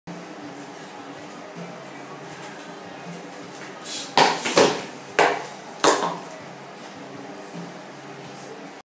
ROME/MMA
Jump inside crescent on a high target without hitting what your not suppose to hit.
PURPOSE: Accuracy, balance, focus, speed, all at the same time.